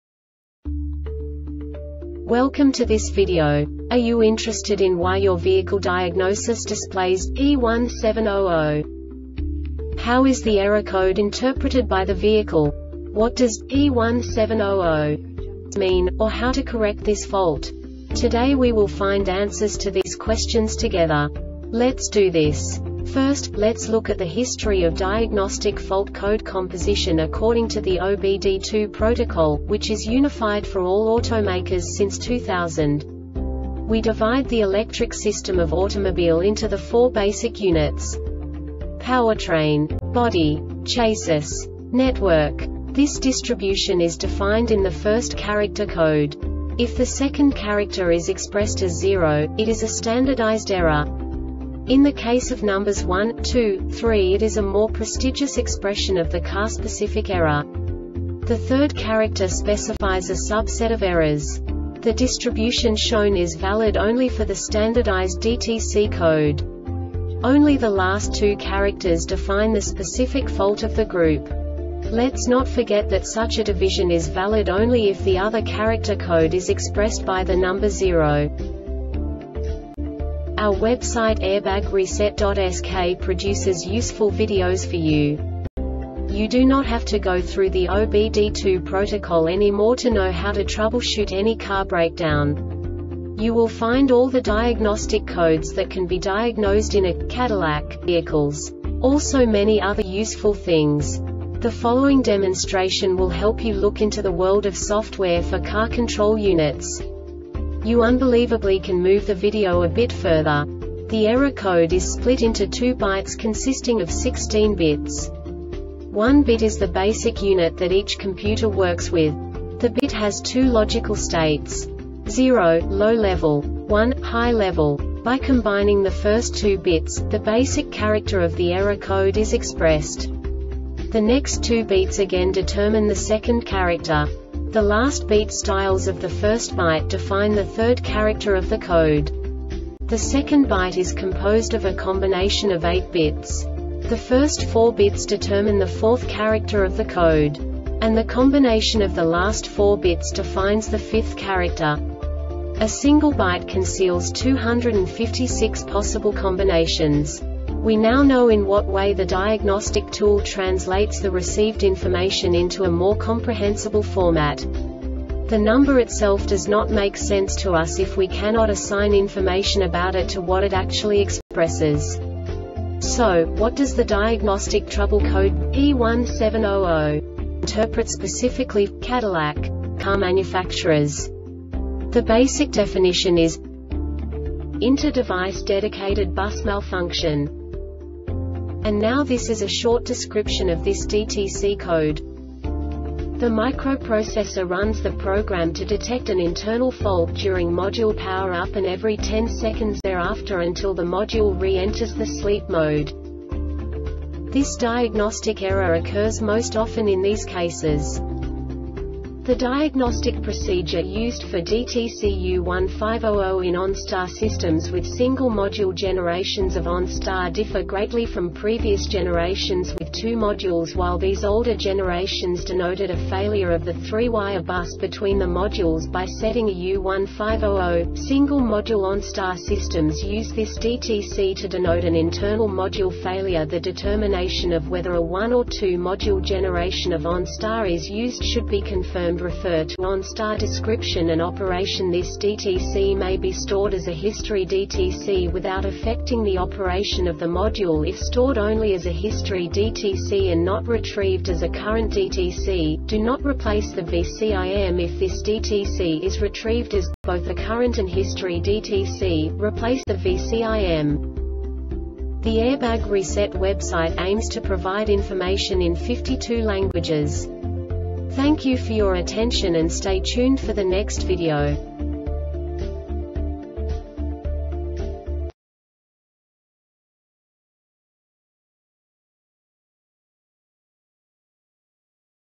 DTC Cadillac P1700 Short Explanation
Contents:
0:21 Basic DTC analysis according to OBD2 protocol standard.
1:48 Insight into programming
2:58 A diagnostically understandable description of the Diagnostic Trouble Code
3:05 Basic error definition

Make:
Cadillac
Code:
P1700
Definition:
Inter-Device Dedicated Bus Malfunction
Description:
The microprocessor runs the program to detect an internal fault during module power up and every 10 seconds thereafter until the module re-enters the sleep mode.
Cause:
 The diagnostic procedure used for DTC U1500 in OnStar® systems with single module generations of OnStar® differ greatly from previous generations with 2 modules.While these older generations denoted a failure of the 3-wire bus between the modules by setting a U1500, single module OnStar® systems use this DTC to denote an internal module failure.The determination of whether a one or 2-module generation of OnStar® is used should be confirmed.Refer to OnStar Description and Operation . This DTC may be stored as a history DTC without affecting the operation of the module.If stored only as a history DTC and not retrieved as a current DTC,do not replace the VCIM. If this DTC is retrieved as both a current and history DTC,replace the VCIM.